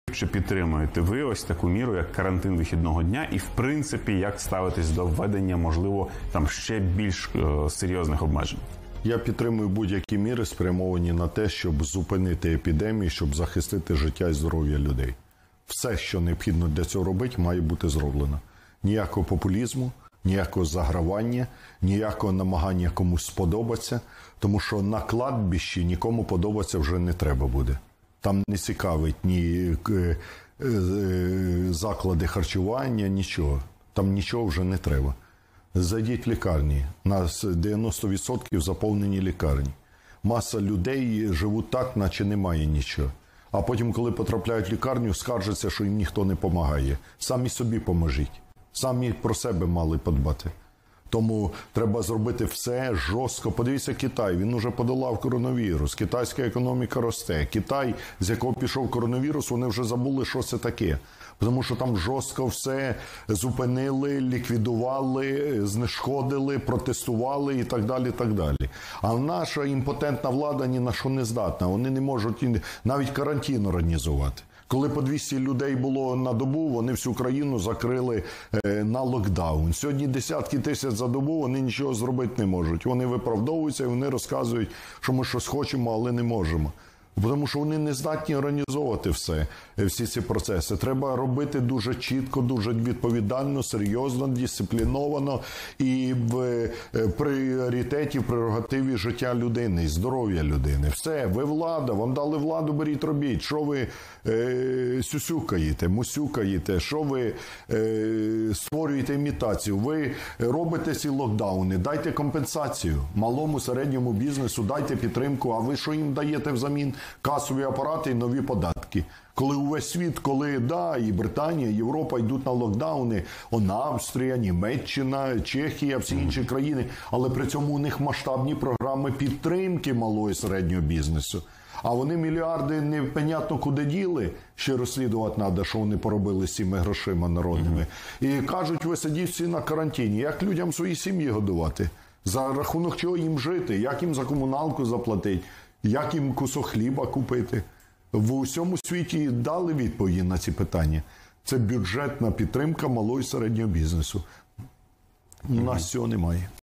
Ляшко-владі: Робите локдаун – дайте компенсацію малому і середньому бізнесу
Локдаун через пандемію коронавірусу має бути тільки з компенсаціями малому і середньому бізнесу. Про це заявив Олег Ляшко в ефірі телеканалу «Україна 24». Лідер Радикальної партії розкритикував владу розкритикував владу за невчасну організацію карантину та нецільове використання коронавірусного фонду.
«Наша імпотентна влада не може навіть карантин організувати. Коли хворіли по 200 чоловік на добу, вони всю країну закрили на локдаун. Сьогодні десятки тисяч хворих на добу - вони нічого зробити не можуть, виправдовуються, що щось хочемо, але не можемо. У пріоритеті має бути життя і здоров'я людини. Ви влада – беріть і робіть. Що ви сюсюкати-мусюкаєте, створюєте імітацію? Робите локдаун – дайте компенсацію малому і середньому бізнесу, дайте підтримку, а ви що робите натомість – касові апарати і нові податки. Як людям свої сім'ї годувати?» - обурився Ляшко.
Лідер Радикальної партії також звернувся до українців та закликав дотримуватись заходів безпеки під час пандемії.
«Я підтримую будь-які заходи, спрямовані на те, щоб зупинити епідемію і захистити життя та здоров'я людей. Ніякого популізму, ніякого загравання, ніякого намагання комусь сподобатися, тому що на кладовищі нікому вже подобатися не треба буде. Там не цікавлять ні заклади харчування, ні щось інше - там нічого вже не треба. Зайдіть в лікарні - вони на 90% заповнені. Маса людей живе так, ніби нічого немає, а потім, коли потрапляють до лікарні, скаржаться, що їм ніхто не допомагає. Самі собі допоможіть. Самі про себе повинні подбати», - сказав Ляшко.